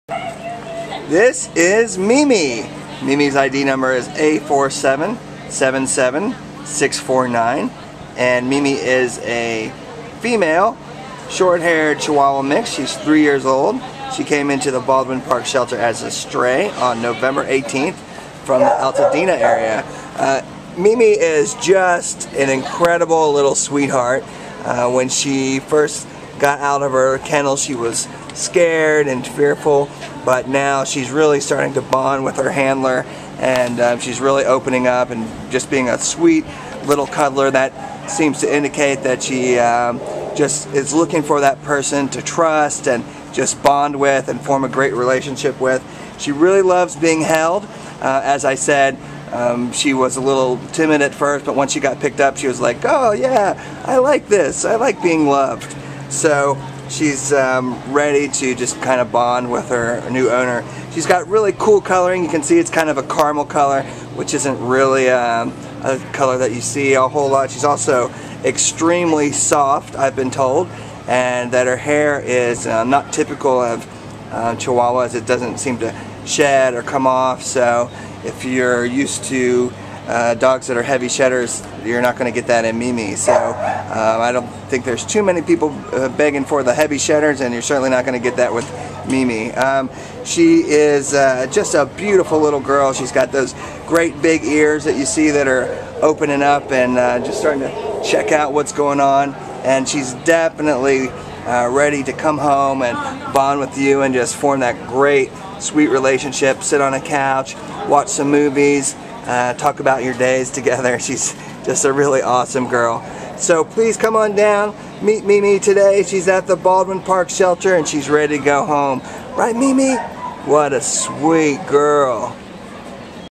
Mimi A4777649
A4777649 Mimi is a soft-spoken 3-year-old white-and-caramel female Chihuahua mix who was found in Altadena on November 18th and brought to the Baldwin Park Animal Care Center. Weighing 6 lbs, Mimi wants nothing more than to be a homebody—to stay at home and be with those she loves. Her physical appearance is quite special, in a subtle, understated way: her hair is very soft, has caramel running through it and dominating on her ears, and looks like it may be the non-shedding type. She also has those wonderful airplane ears, and a delightful squint at times, as if she’s a bit delicate for the goings-on. She loves being held and carried but also walks well on leash, and she gets along with her kennel mates, who are her size. She will do best as an indoor pet who can focus on loving and being loved by her person or family; any children should be old enough to respect her small size. She’ll do well anywhere—in an apartment, condo, or single-family home.